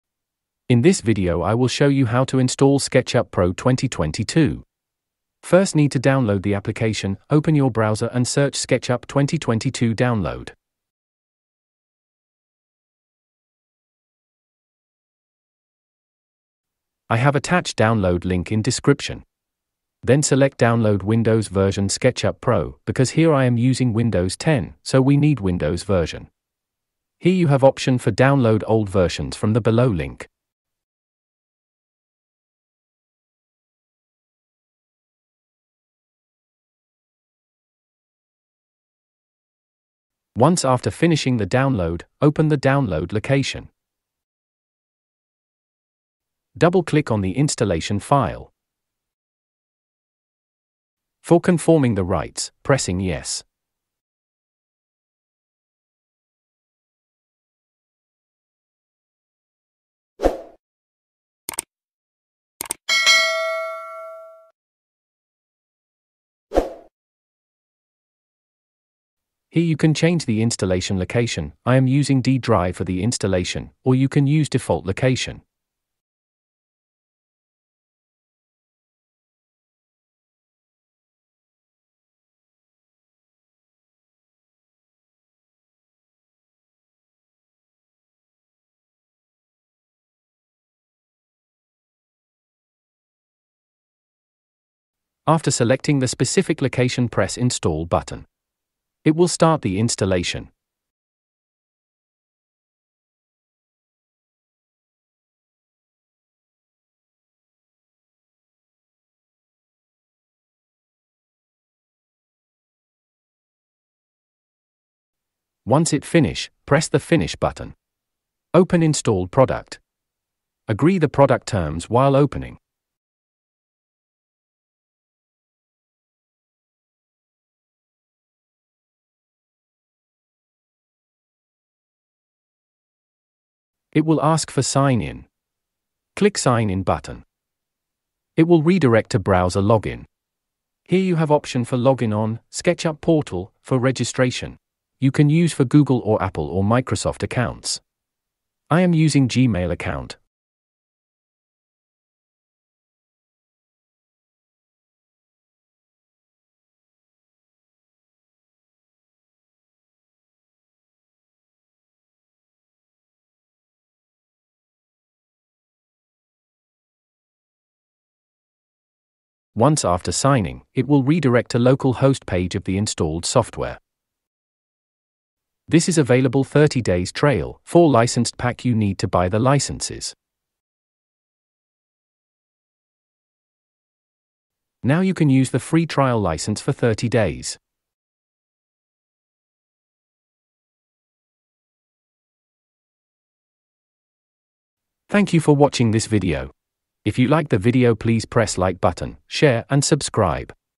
sketchup pro install | Install sketchup 2022
This video will help to downlaod and install sketchup2022 , This will help to know easy way of downloading sketchup and activation of sketchuponline

Details of Install sketchup2022 :-
00:00 Introduction of sketchup installation
00:19 Downloading of Sketchup from portal
00:42 Installation of Sketchup2022
01:10 Installation path selection of sketchup pro
02:13 Login sketchup portal for registration
03:21 Outro